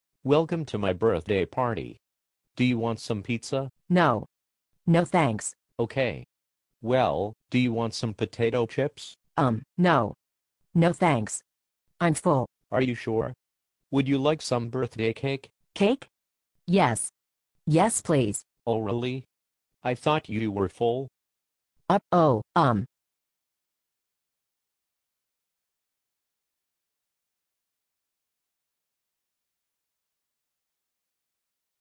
Do you want some of this?
Language Practice:
Do you want some?
Would you like some?
Accept & Decline